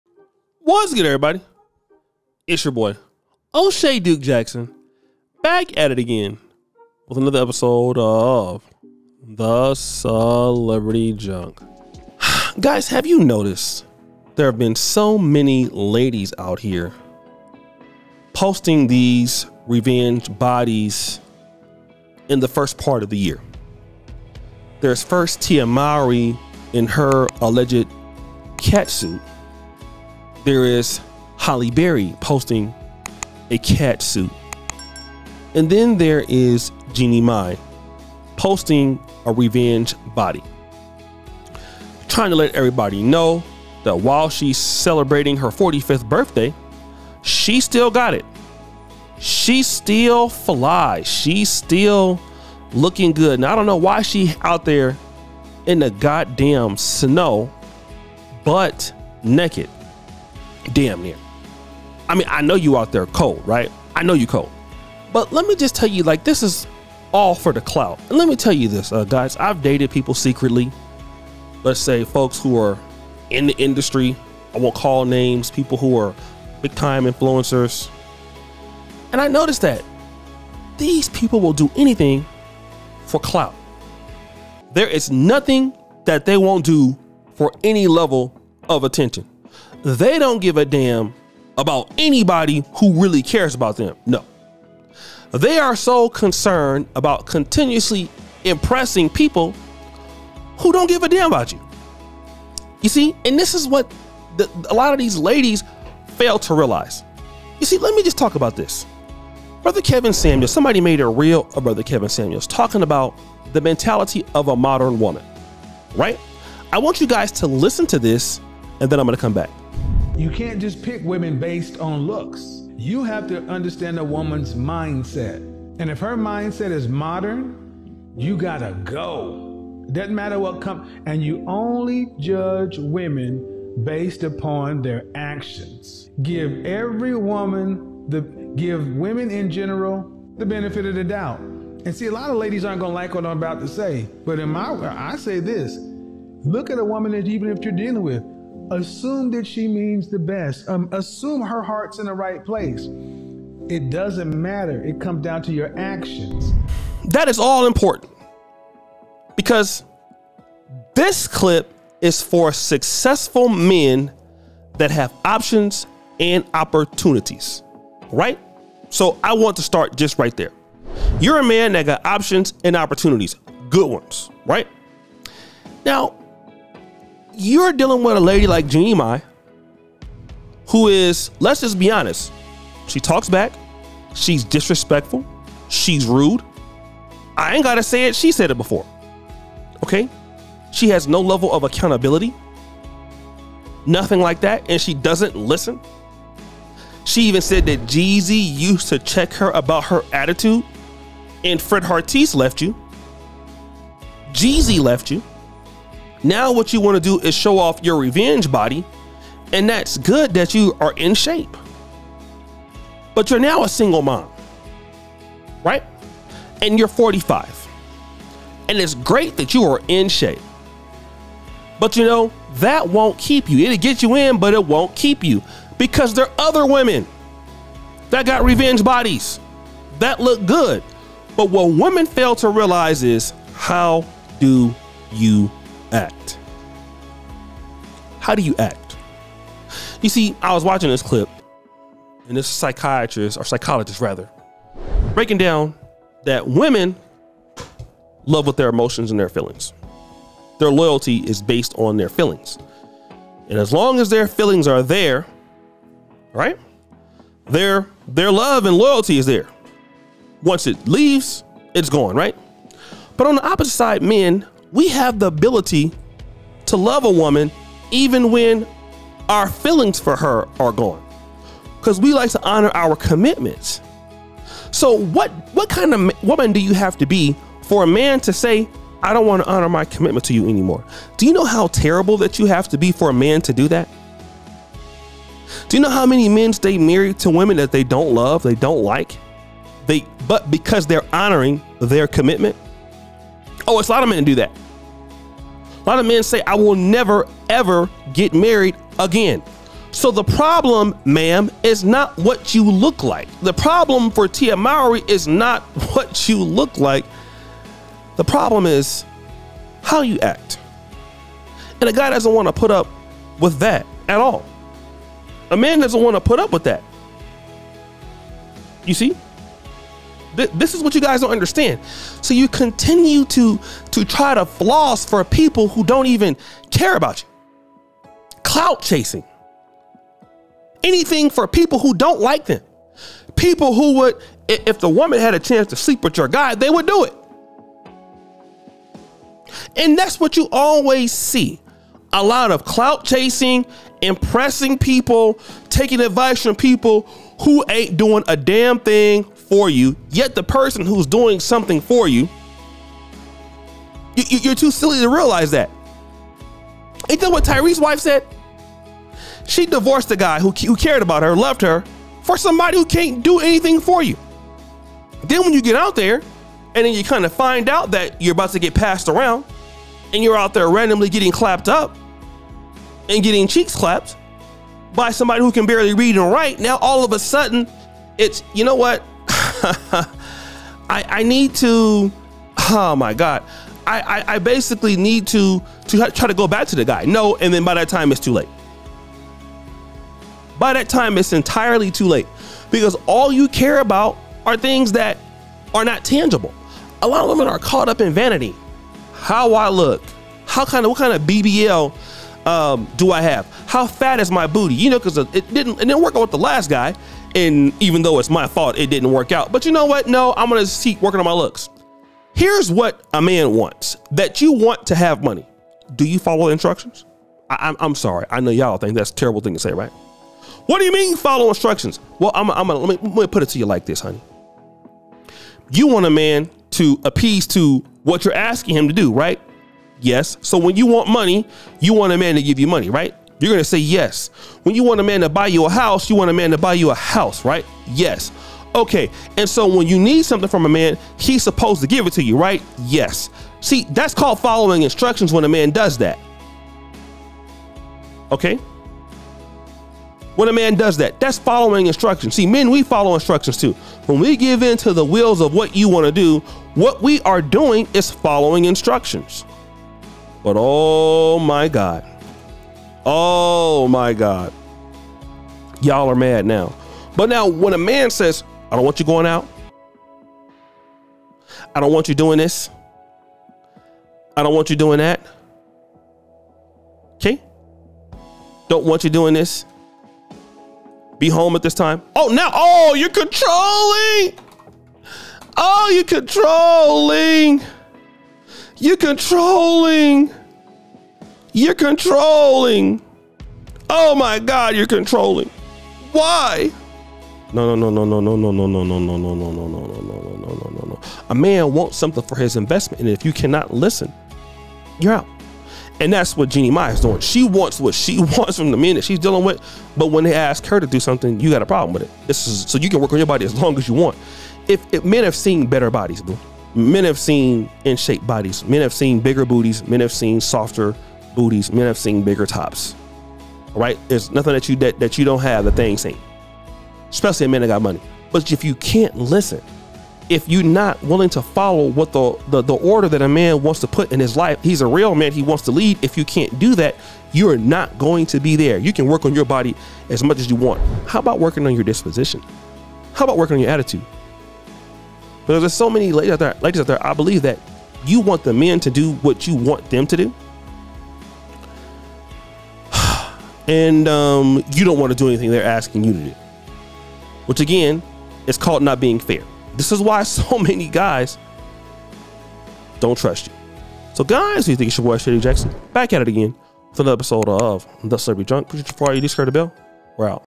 Jeannie Mai is Doing THIS For Clout after Being DUMPED BY JEEZY!
Jeannie Mai cannot stop trying to clout chase after she has been dumped (divorced) 2 times in the last 5 years.

Jeannie Mai is showing off her assets on her 45th birthday.

The TV host rang in her special day Thursday by sharing a series of sizzling Instagram snapshots of herself rocking a metallic gold bikini and matching snow boots during a winter getaway.

She flaunted her toned physique as she posed on a snow-covered dock at a stunning lakeside cabin.

She complemented her bikini with a beige fur coat, a matching beanie and a brown Louis Vuitton fanny pack.

The birthday carousel also included a video of Mai raising a glass of what appeared to be champagne to the camera with a huge grin on her face.

In case fans were wondering why she wore so little in cold temperatures, the “Real” alum playfully captioned the post, “Cuz she can ✨🎂🥳🥳.”